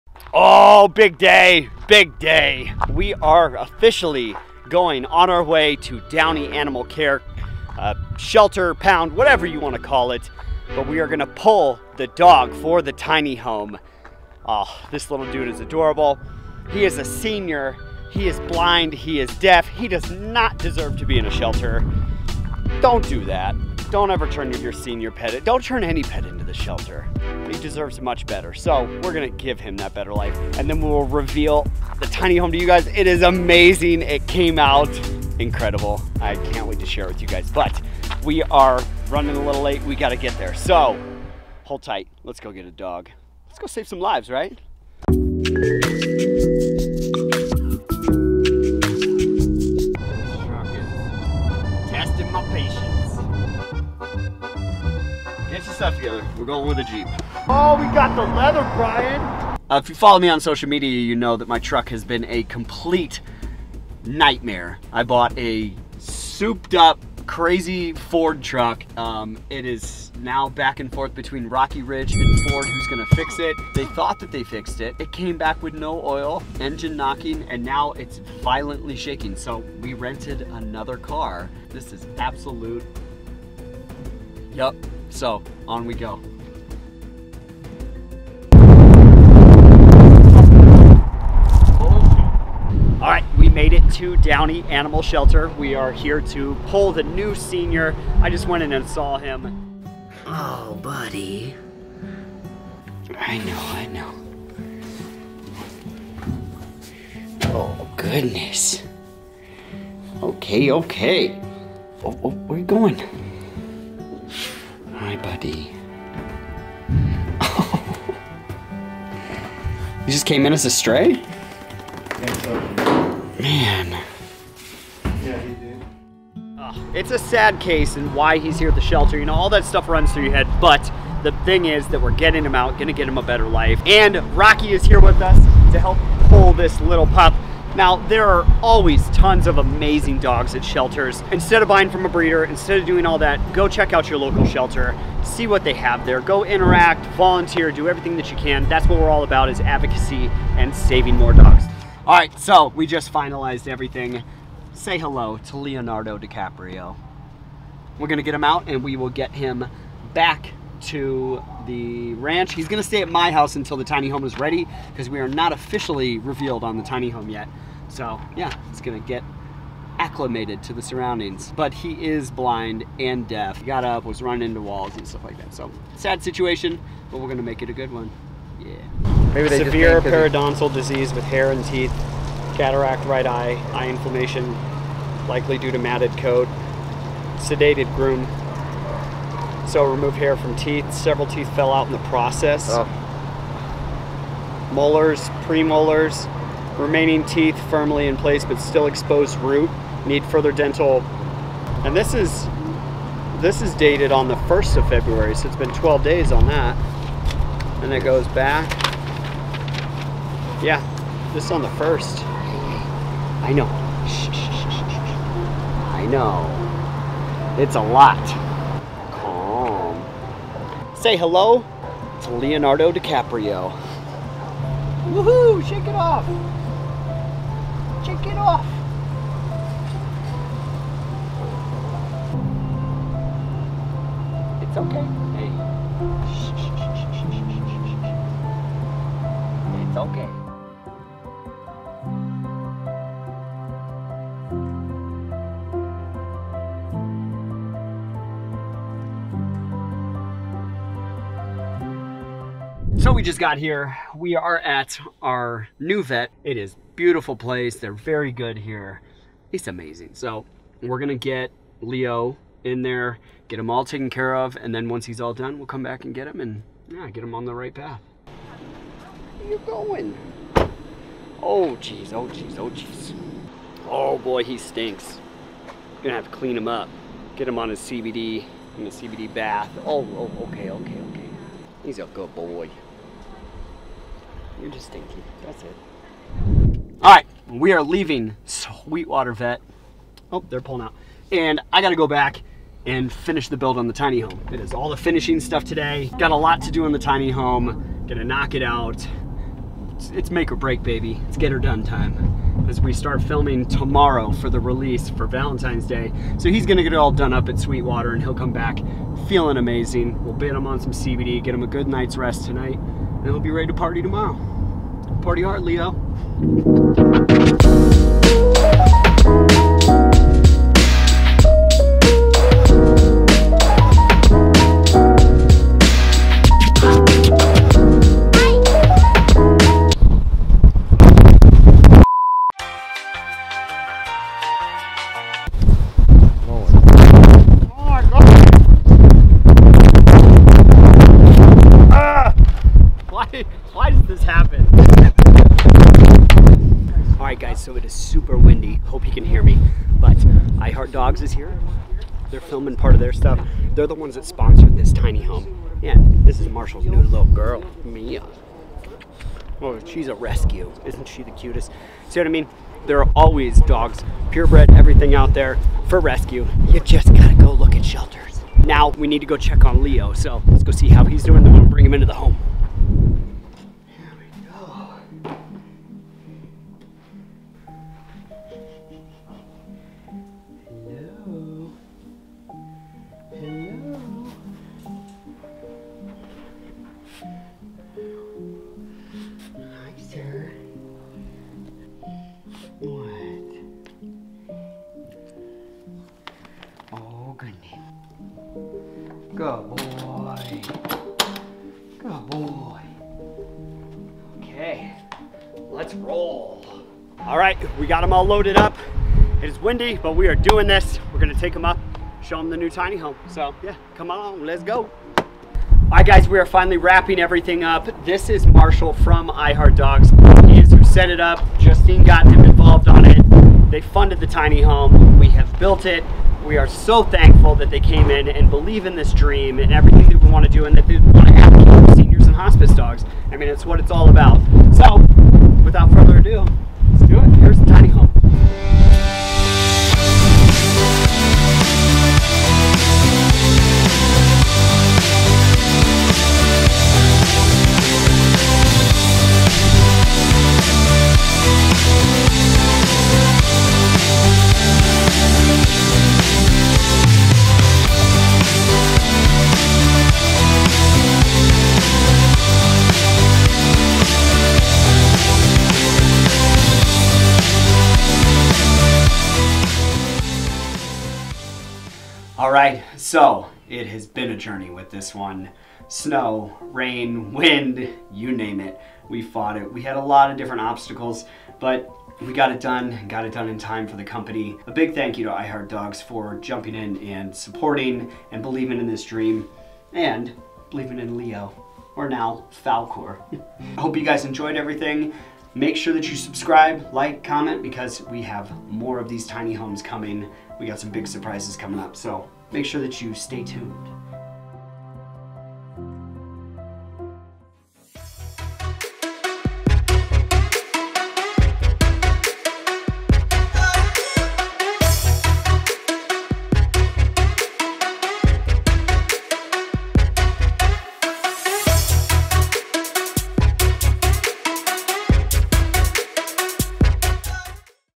Final Reveal of the Tiny Home for a Blind and Deaf Senior Dog - Senior Neighborhood Episode 8
In this video you can come along with us as we go rescue Leo from the shelter and be there for the final release of the finished tiny home we built for him!

Address for sending gifts/letters:
Tye Friis
9001 Elizabeth Lake Road